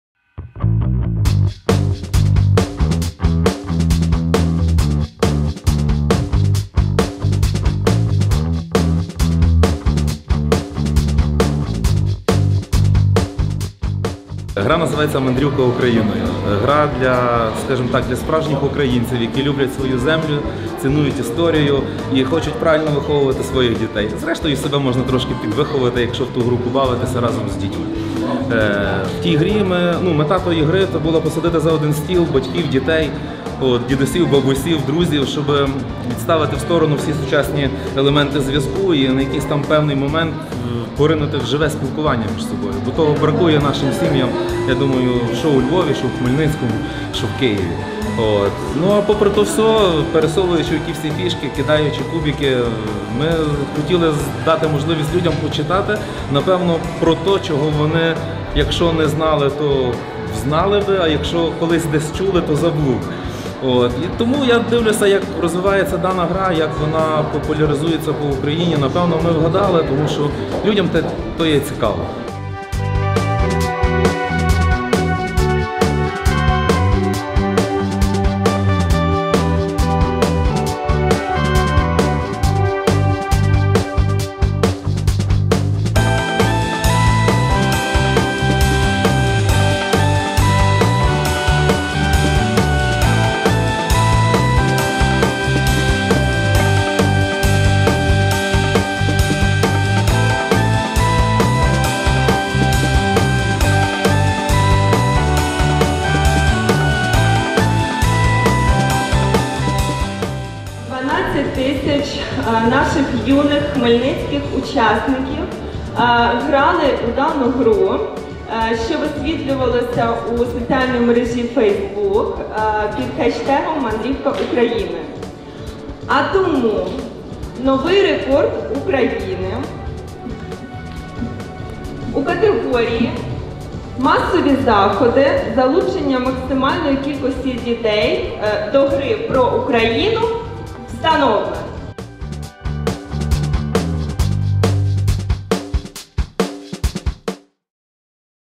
Гра, яка увійшла до Книги рекордів України / "Мандрівка Україною" / Хмельницький
Протягом лютого цього року хмельницькі школярі грали у настільну гру «Мандрівка Україною», публікуючи фото та відео з цих ігор в мережі під хештегом мандрівкаУкраїною. Сьогодні ж, 12 березня, у Палаці творчості та юнацтва підбили підсумки цього флешмобу. У ньому взяли участь 30 шкіл міста і понад 12 тисяч учасників. завдяки акції у Хмельницькому був встановлений новий рекорд України у категорії «Масові заходи» із залученням максимальної кількості дітей до гри про Україну.